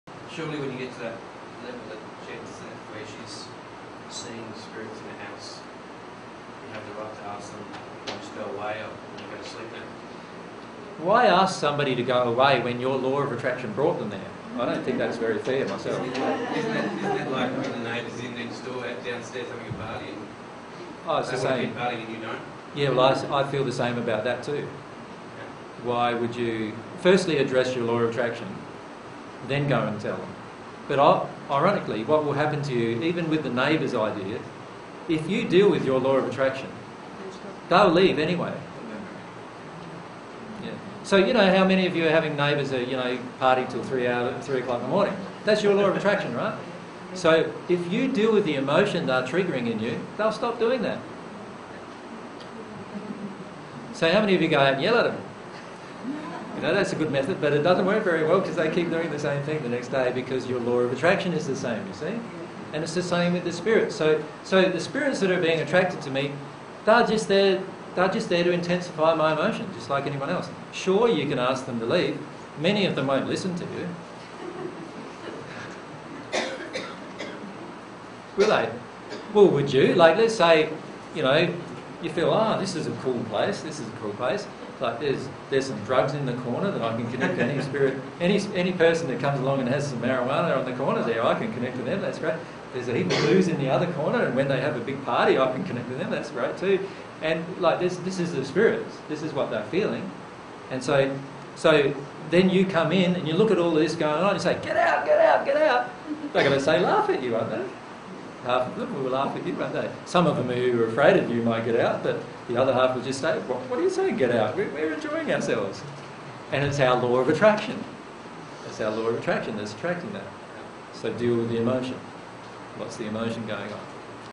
Jesus on How to Change Events and Spirits We Attract
Jesus talks about how events and spirit attractions change permanently when we deal with the emotions causing the attraction.

This is a section of the presentation by Jesus entitled "20090208 Spirit Relationships - The Law of Rapport & Spirit Communication P2".